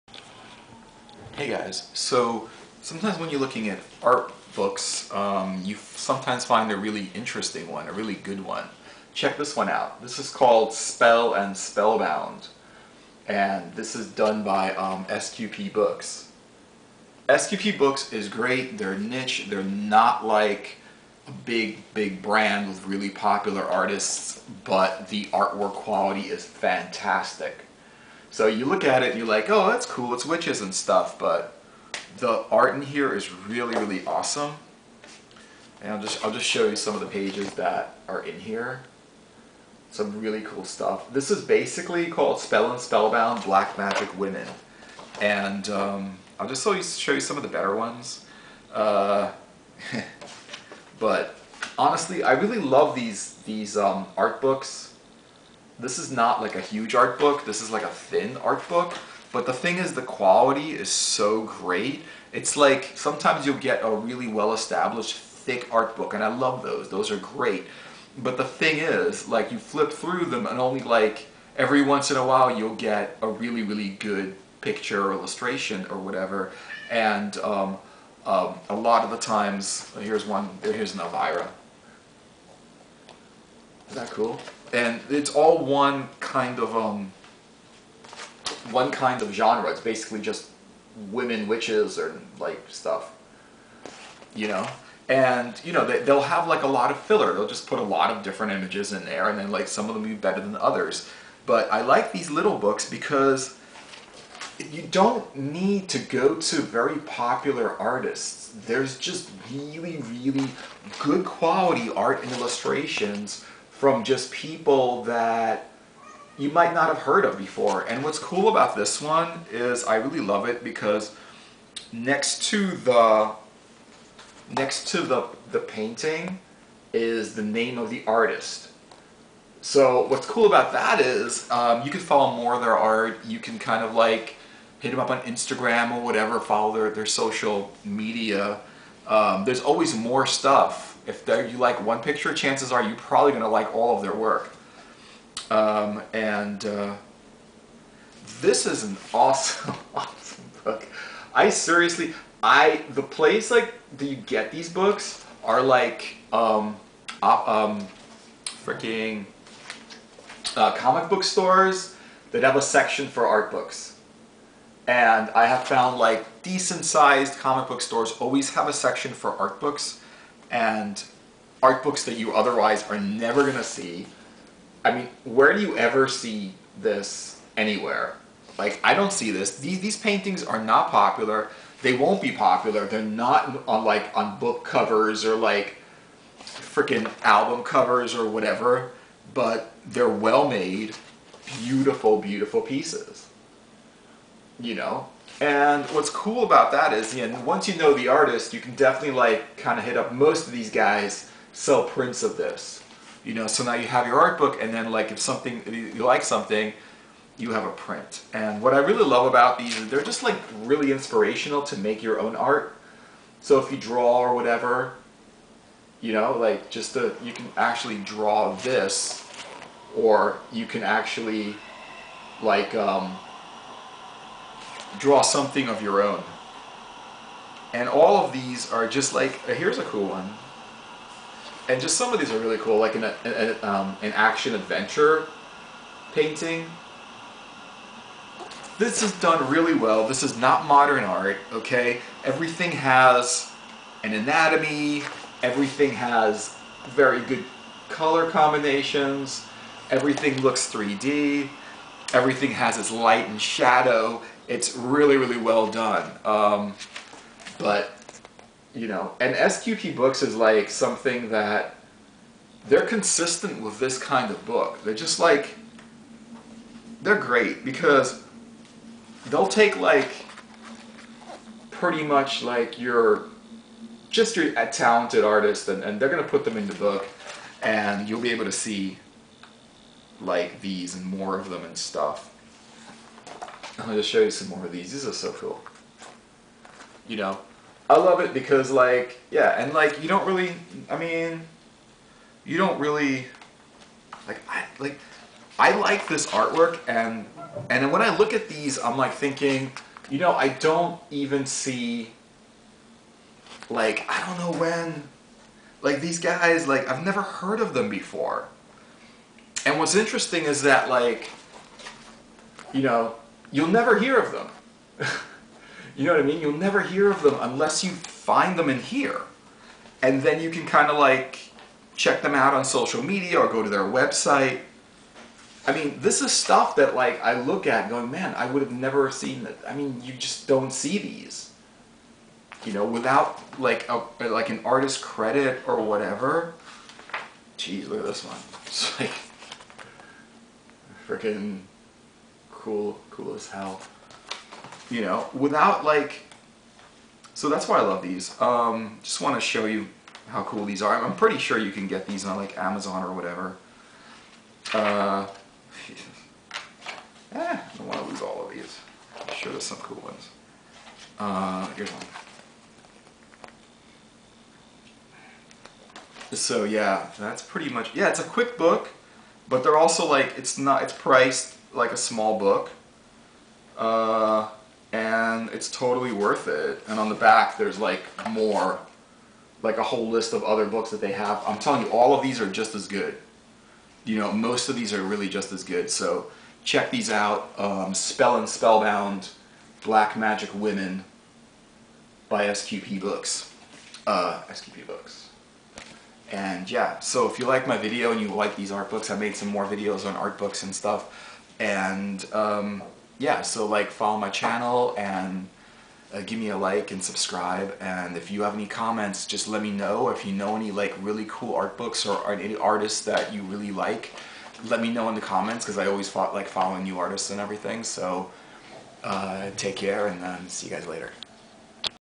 "Spellbound. Black magic women". Art book review
This is the art of Female witches and magicians painted by various artists.